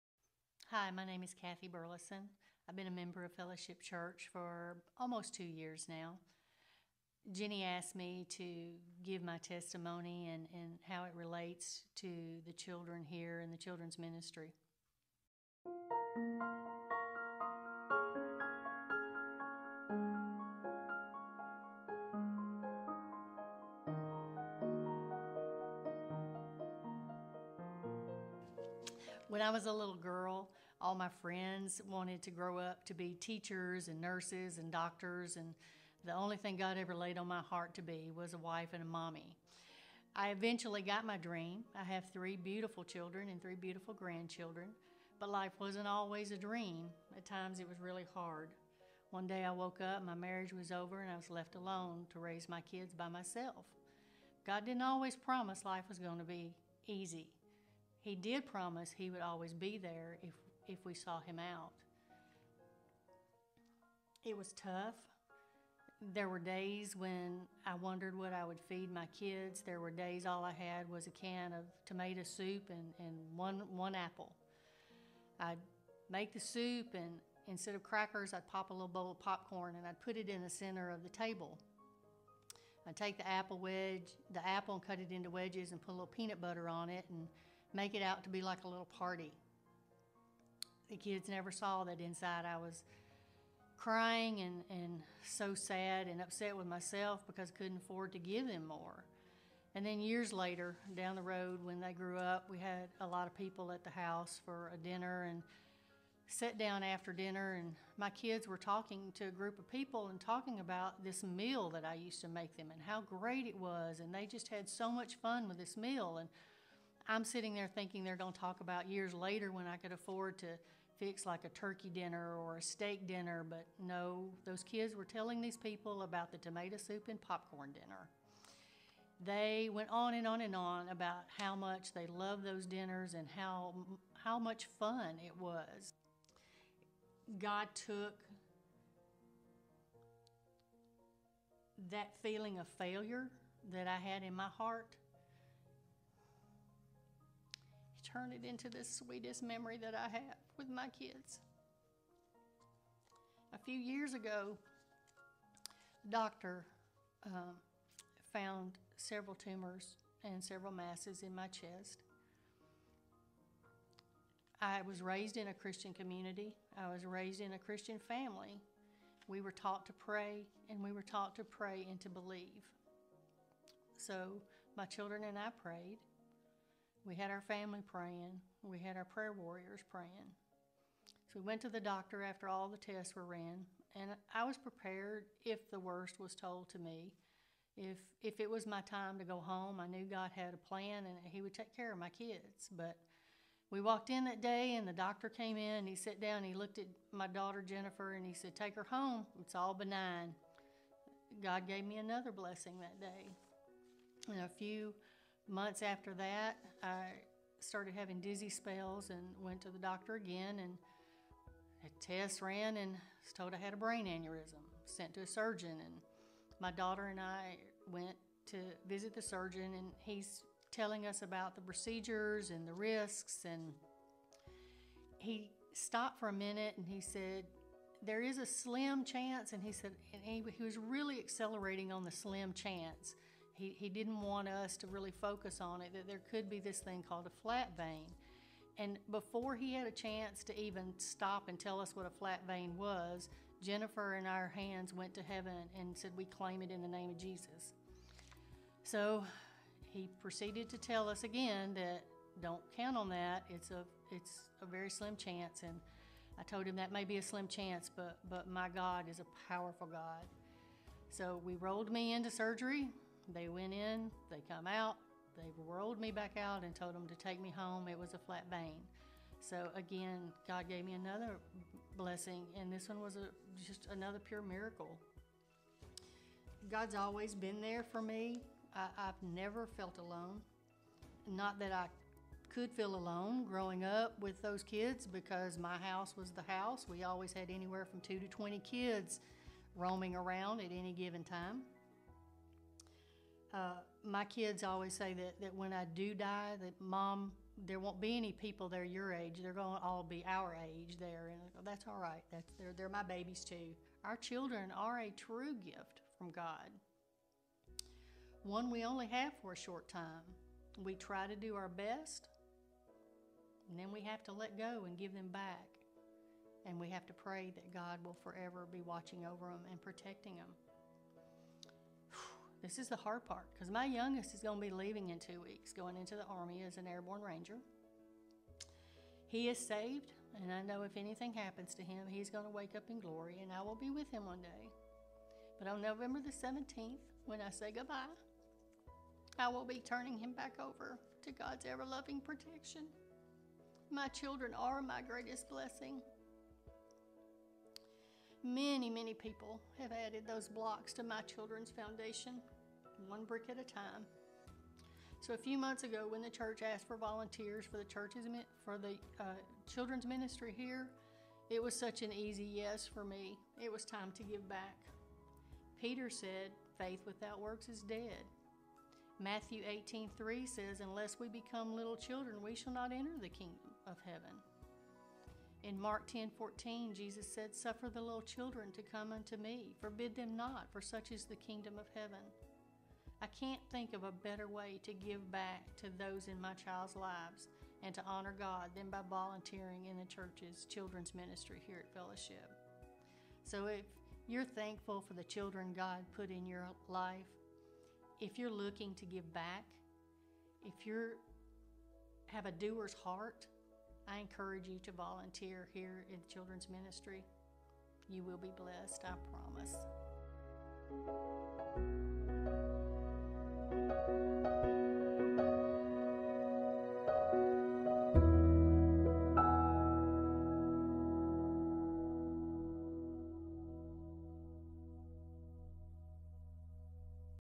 Fellowship Church Murfreesboro | Kathy Burleson's Testimony on Gods Healing Power
A story of God's faithfulness and healing find out more at Fellowship Church in Murfreesboro